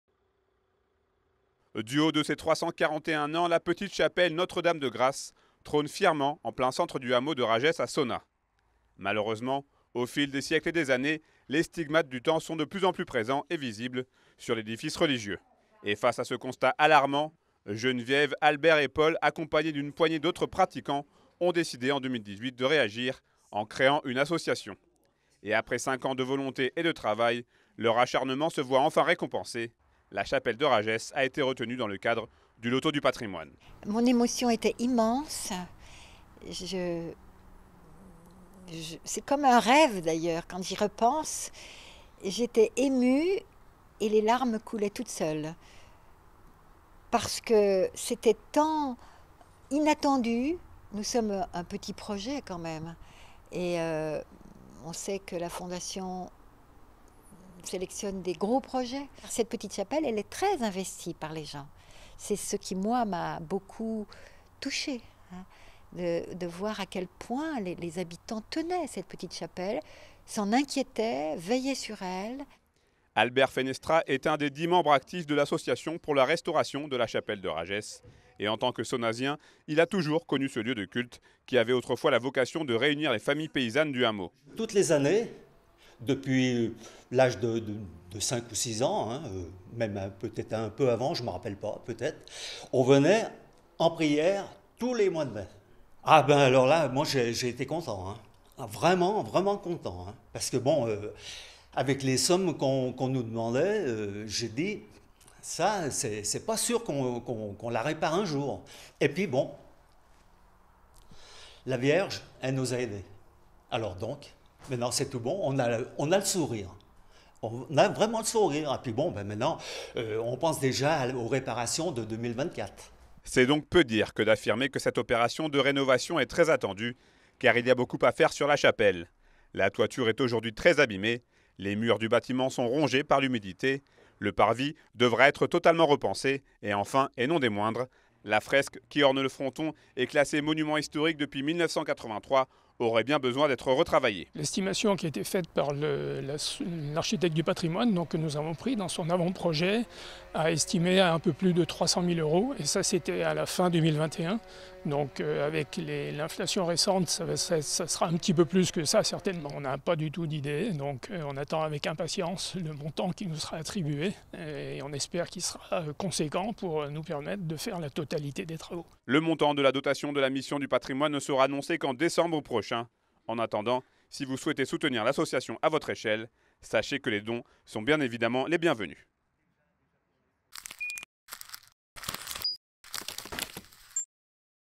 La Chapelle de Ragès va pouvoir se refaire une beauté
En Savoie, dans le hameau de Ragès à Sonnaz, une association a enfin vu son rêve s’exaucer. Son projet de restauration de la petite chapelle du village a été retenu par la Mission Stéphane Bern pour le loto du patrimoine. Reportage de Sébastien Germain.